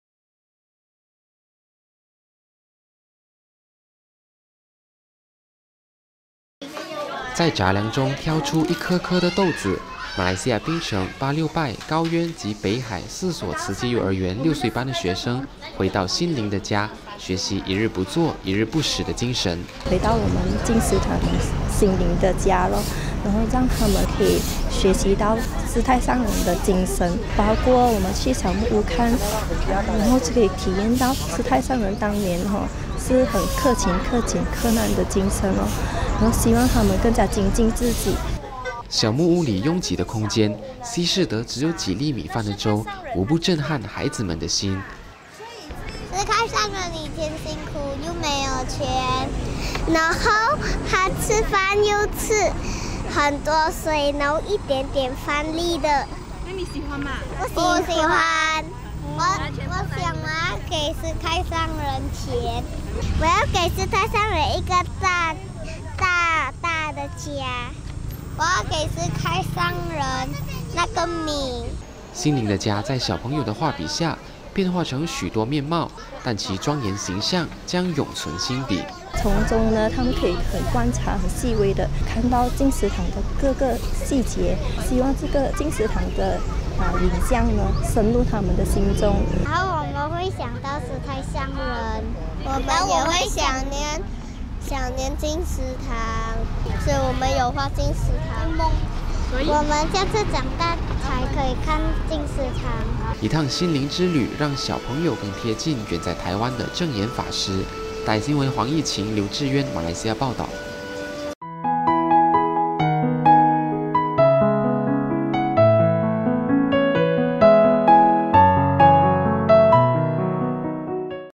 20180508【教育】幼兒回歸心靈的家
馬來西亞檳城，峇六拜，高淵及北海四所慈濟幼兒園的小朋友回歸心靈的家。這一趟旅程，小朋友彷彿去到台灣，和證嚴法師的心更靠近一些。